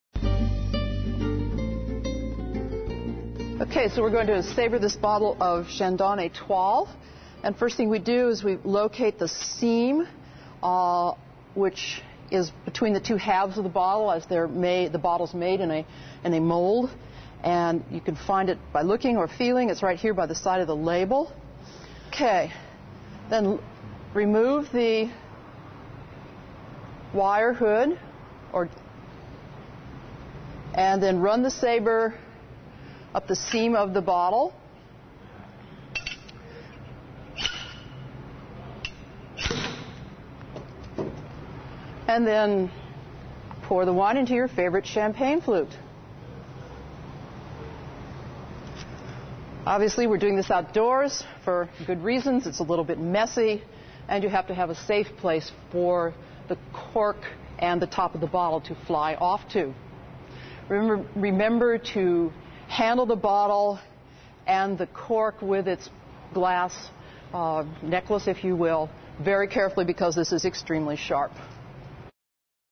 Wine Tasting Program
Professor Marian Baldy has taken advantage of WebCT course component technology and video production from IMC to provide her students in Physical Science 195 Wine Tasting with lectures reinforced by PowerPoint and videos. The videos provide concise information and demonstrations of wine making, such as Sabering Champagne, Information about Vineyards, Aroma Wheel Chart, Opening the Wine Bottle, Wine Tasting Impressions by Sight, Bouquet and Tasting, and an Approach to Wine Sensory Evaluation.